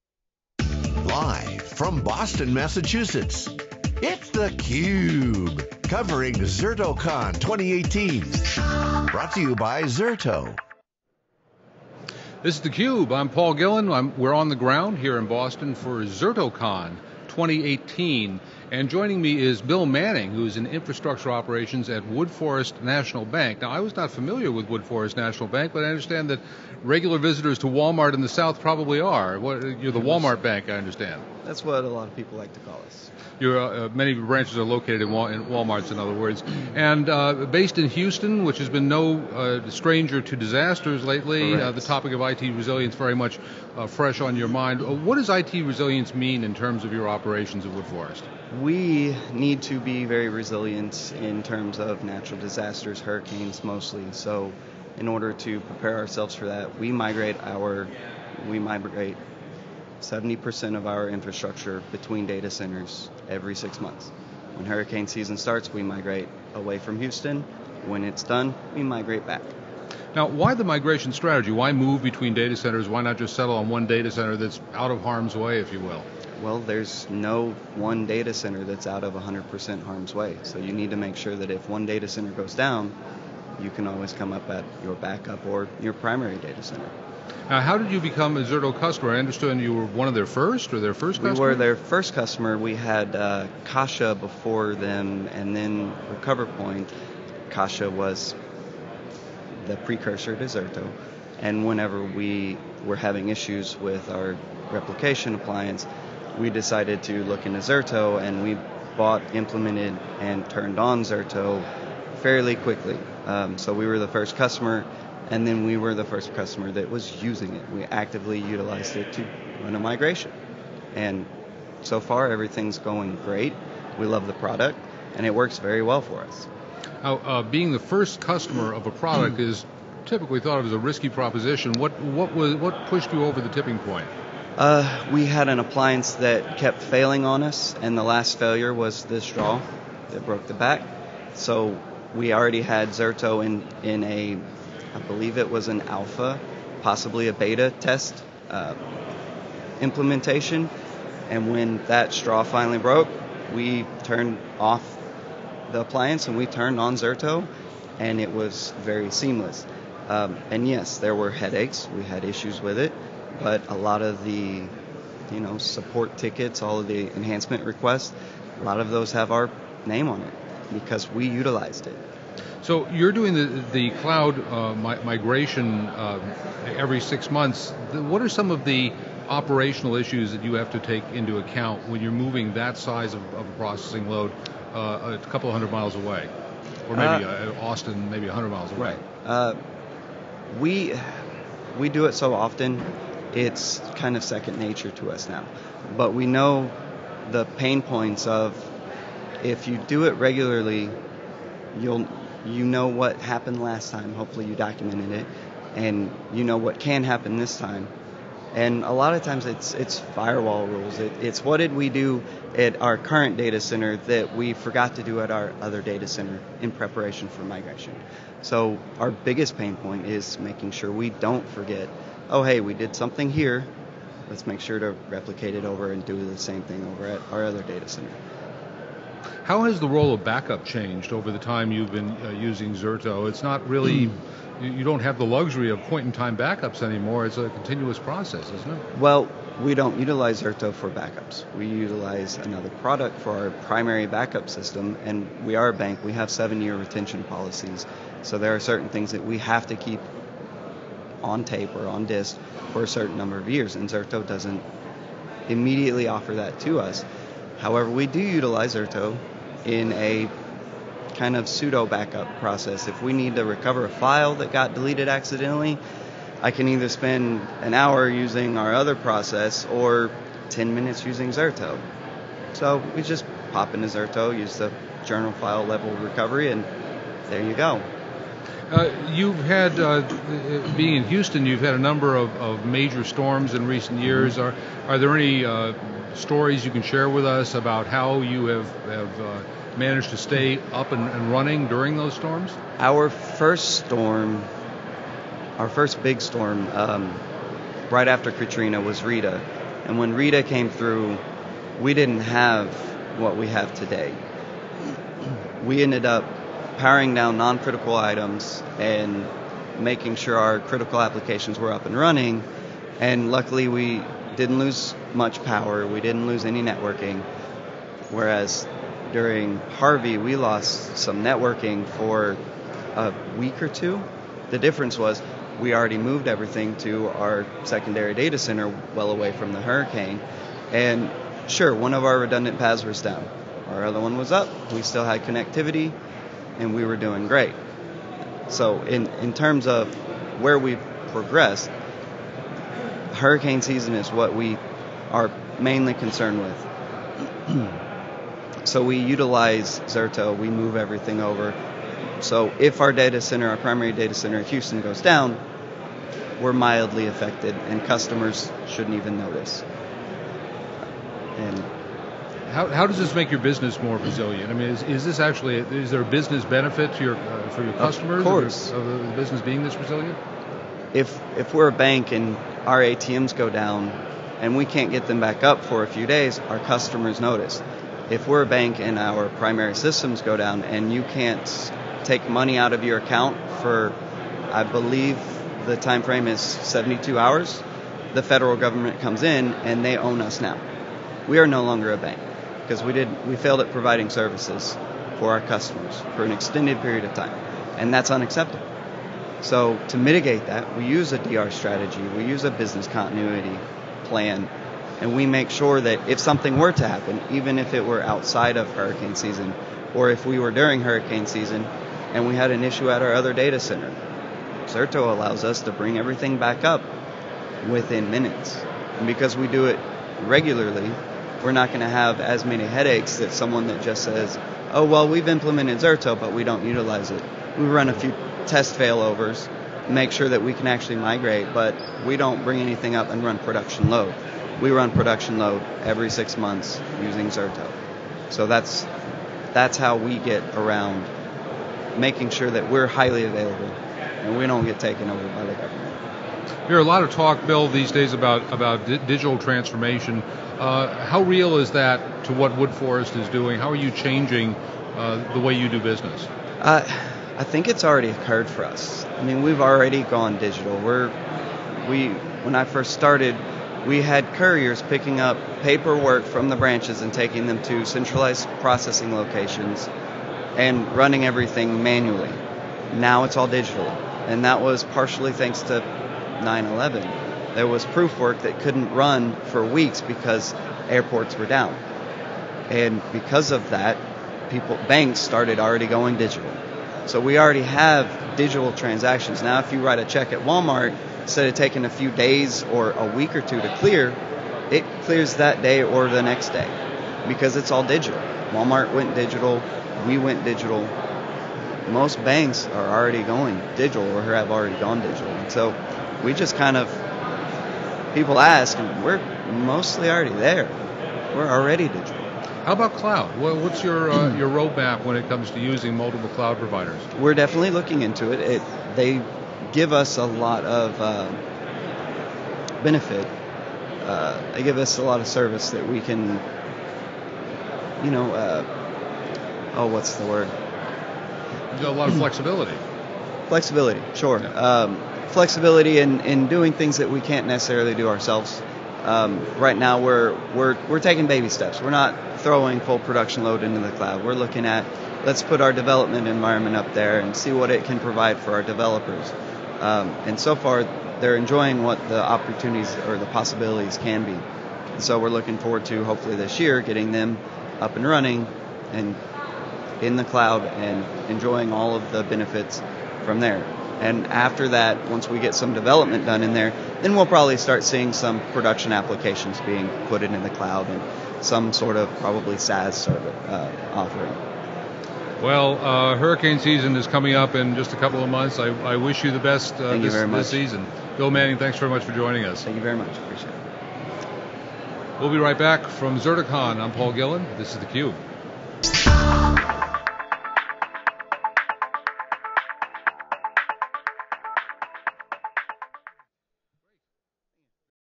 Bill Manning, Woodforest National Bank | ZertoCON 2018
Bill Manning, Infrastructure Operations at Woodforest National Bank, sits down with theCUBE's Paul Gillin at ZertoCON 2018, from Hynes Convention Center in Boston, MA.